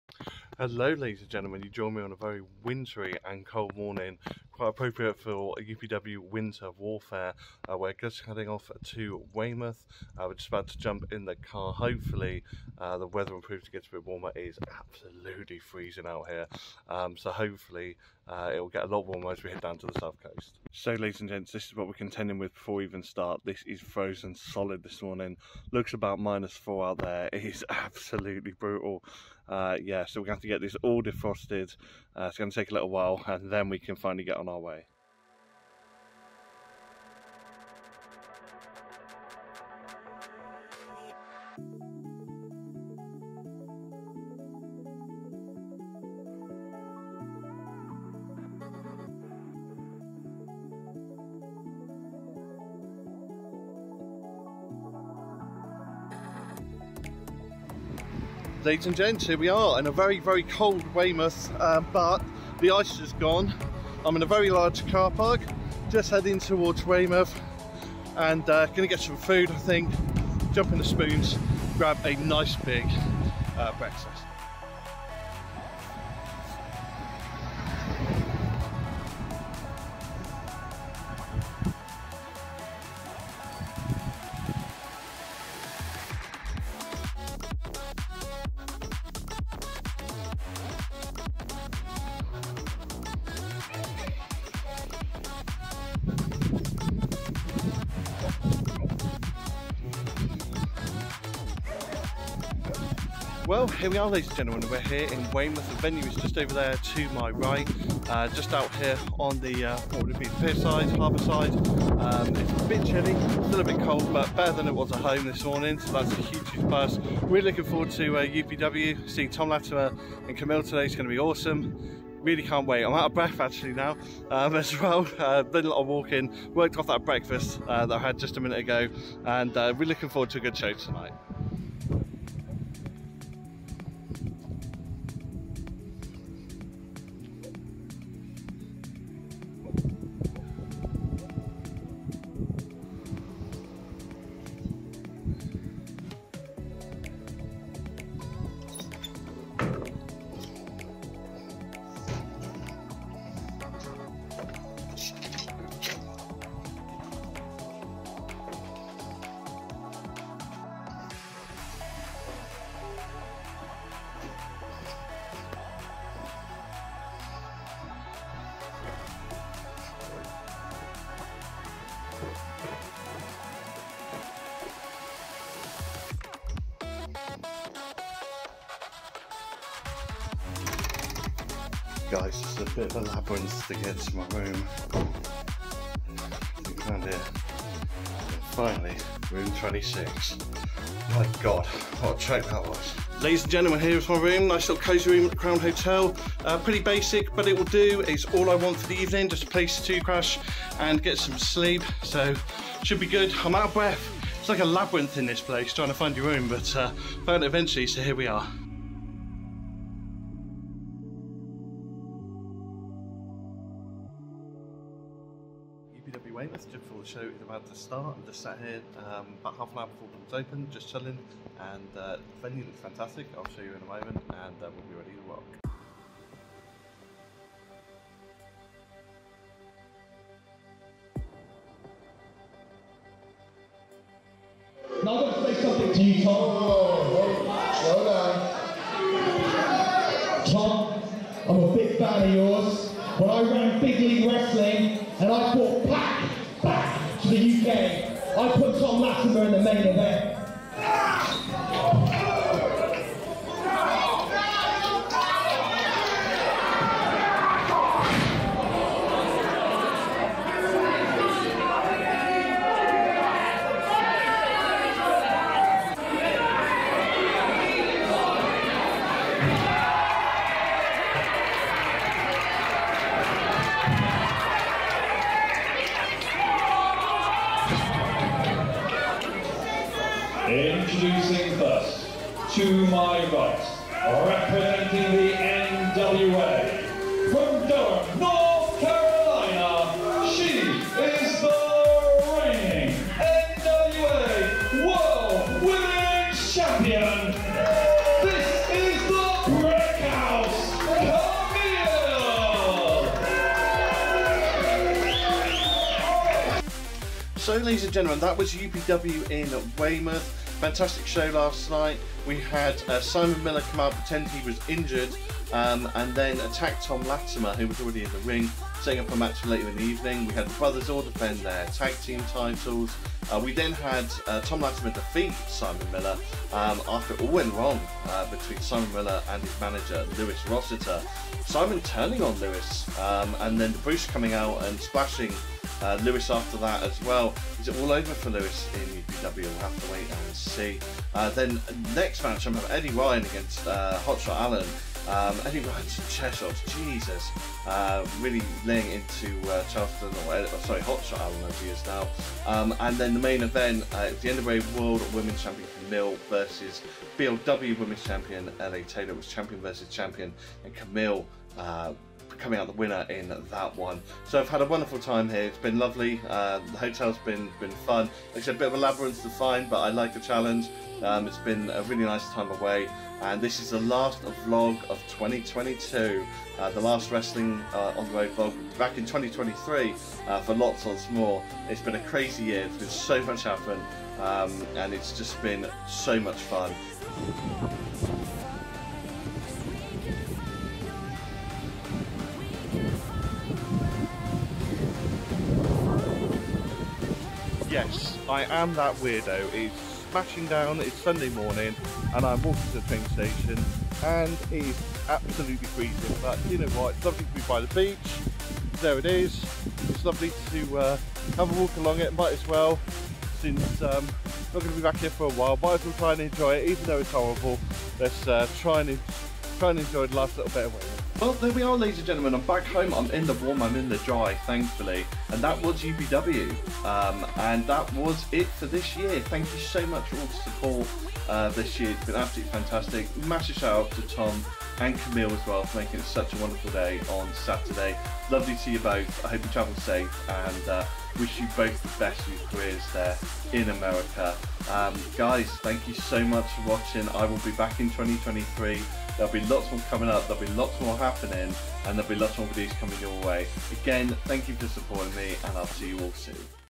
The Wrestling vlog #14 - Warfare in Weymouth with NWA ThomLatimer and Kamille
The Wrestling Vlog #14 UPW Winter Warfare With Thom Latimer and Kamille on the show, Simon Miller, and many more!